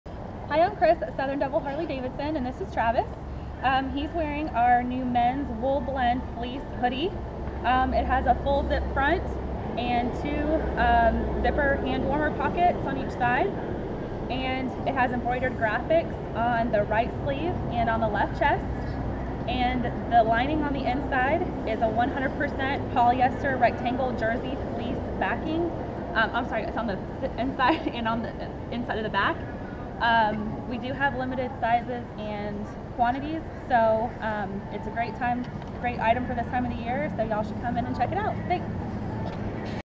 Wool Blend Fleece Hoodie - Travis and Chris
96064-16VM

A fiery shade of brick red, our Wool-Blend Fleece Hoodie arrives in time for the colder weather. We suggest it as a lightweight, but warm, layer over long or short sleeve tees. For a punch of color, we sewed sporty contrasting seams on this comfortable men's hoodie. Peek at the inside for a patterned interior. Zipper pockets keep keys and phone in place.
85% acrylic, 15% wool with bonded 100% polyester rectangle jersey fleece backing
Zipper front
Two zipper hand-warmer pockets
Contrasting seams throughout
Embroidered graphics on right sleeve and left chest